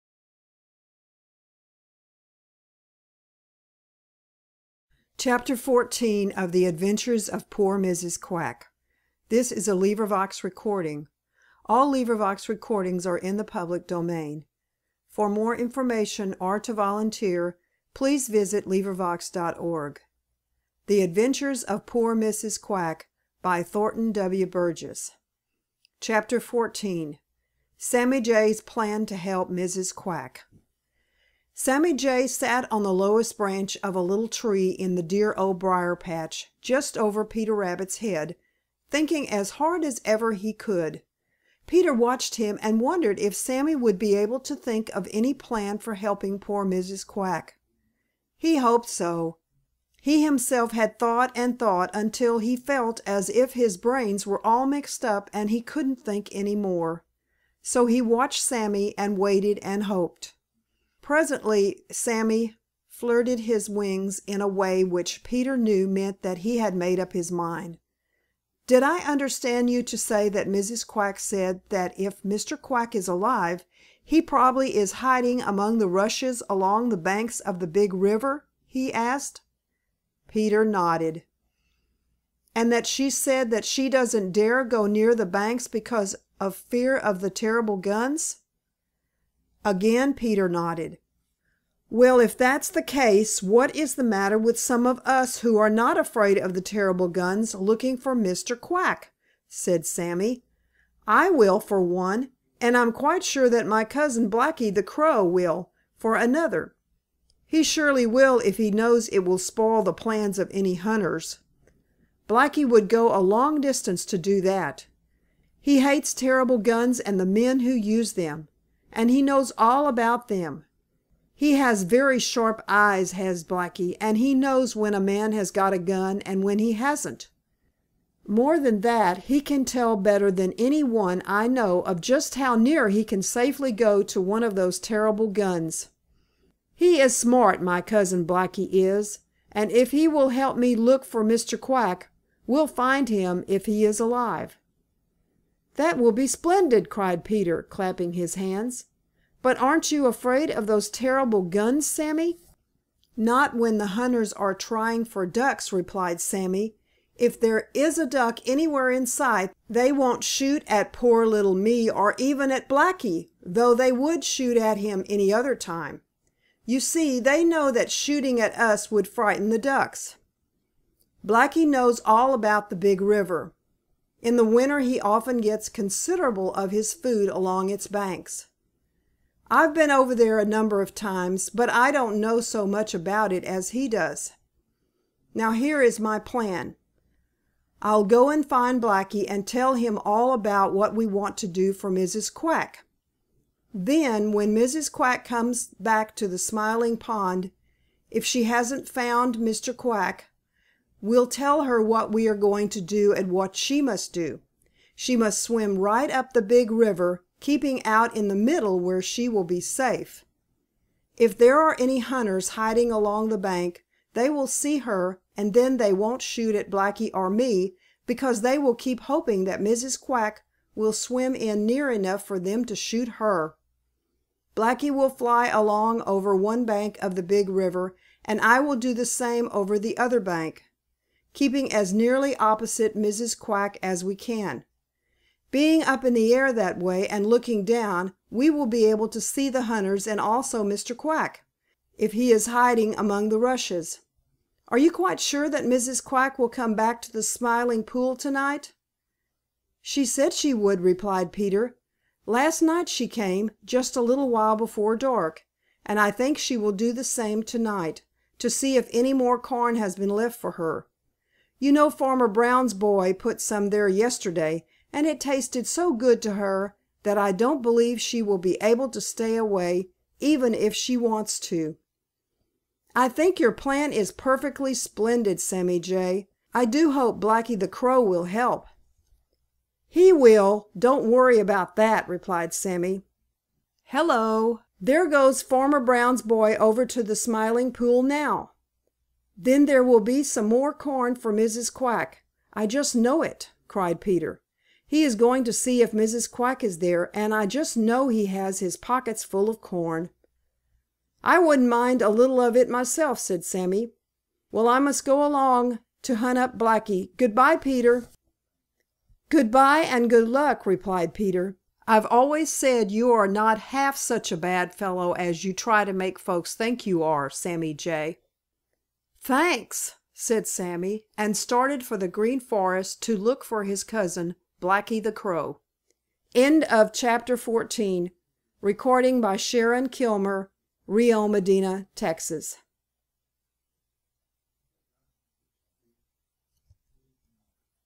The Adventures of Poor Mrs. Quack by Thornton W. Burgess - 14. Sammy Jay's Plan to Help Mrs. Quack
LibriVox recording of The Adventures of Poor Mrs. Quack, by Thornton W. Burgess.

Cover picture book cover. Copyright expired in U.S., Canada, EU. and all countries with author's life +70 yrs laws. Cover design by annise. This design is in the public domain.